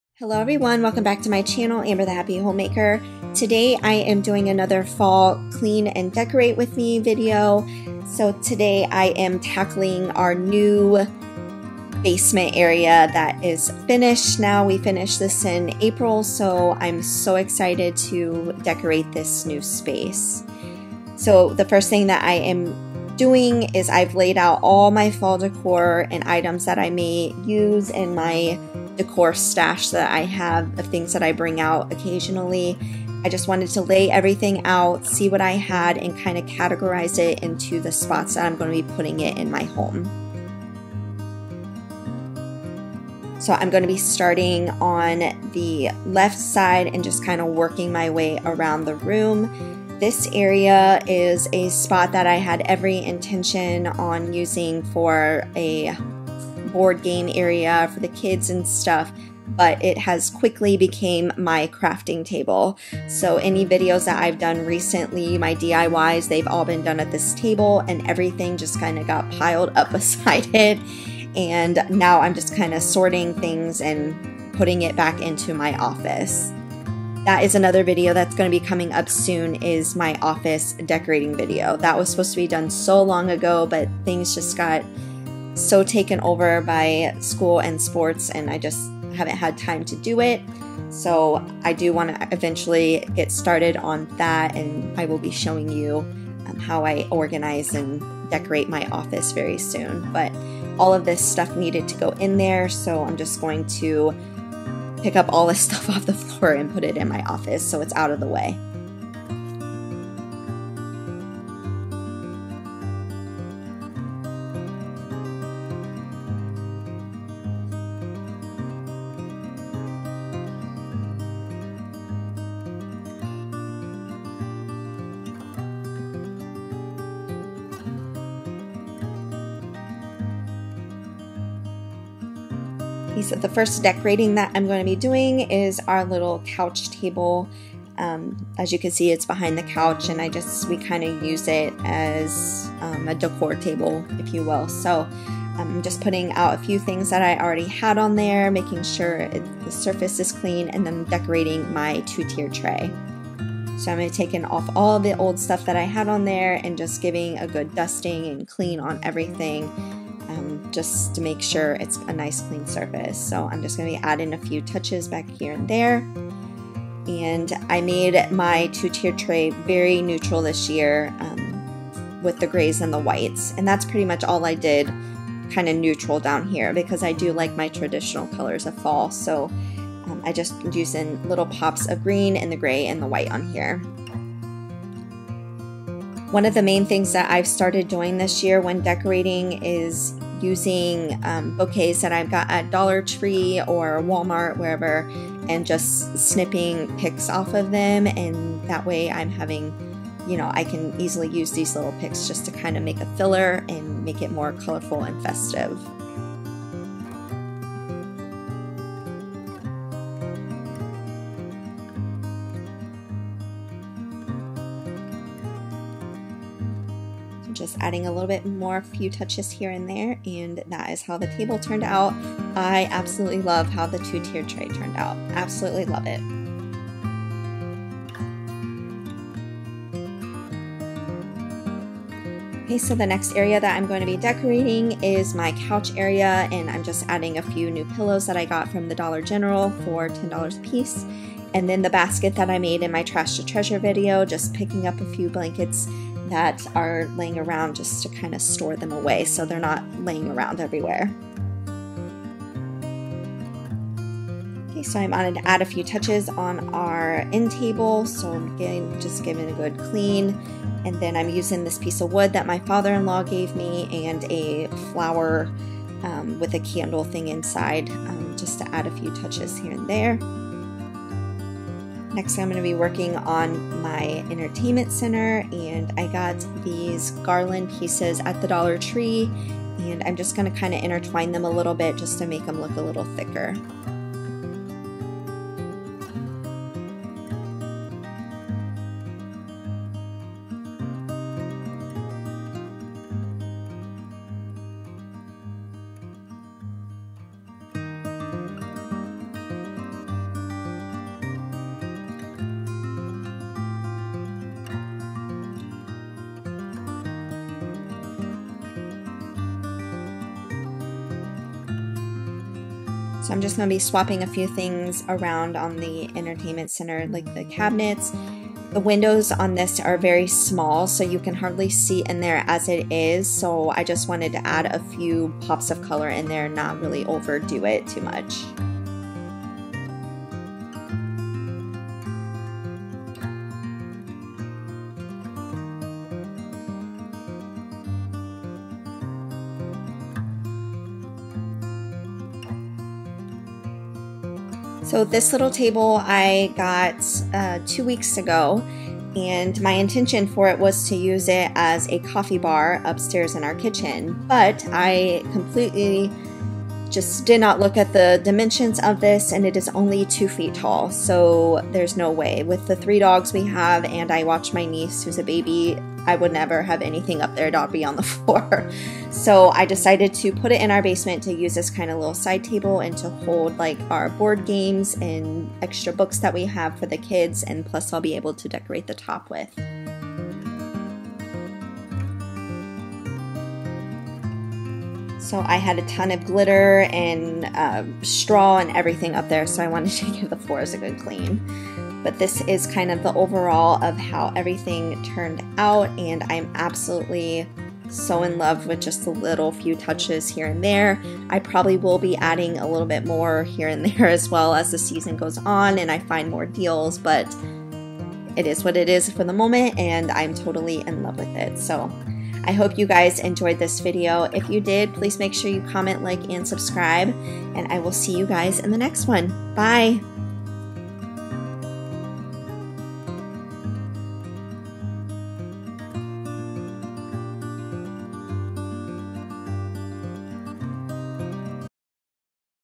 🍁FALL CLEAN AND DECORATE | FALL CLEAN AND DECORATE WITH ME 2019 | FALL DECORATING IDEAS🍁
Hello everyone! Are you getting ready for fall and Halloween in your home? We’re in full force here! Today I bring to you a fall clean and decorate! I love decorating for fall. it’s my absolute favorite! I hope you enjoy this fall clean and decorate with me 2019 and get all the fall decorating ideas!! 😊

Amber